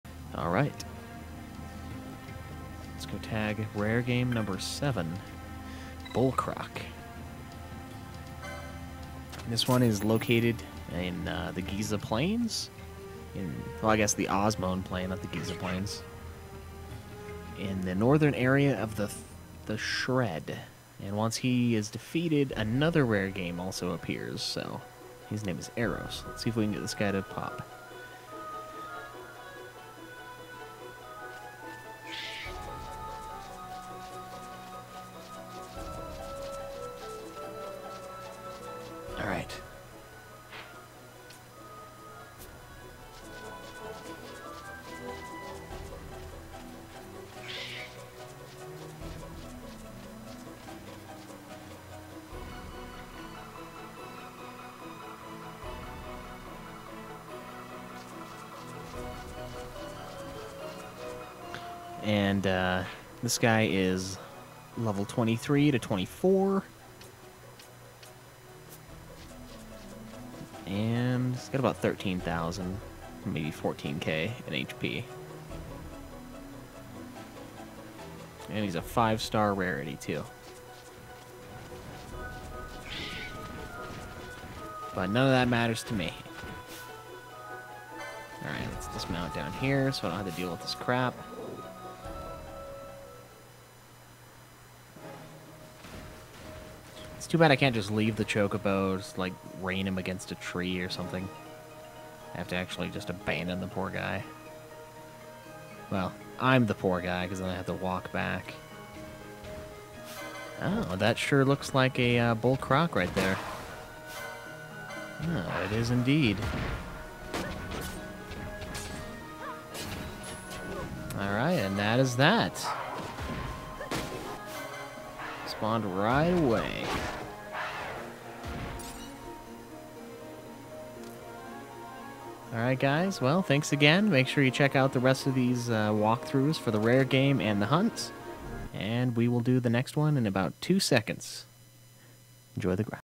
Rare Game No.299: Bull Croc
Kaleb takes on the rare game Bull Croc. He is located in the Shred of the Ozmone Plain. Enjoy!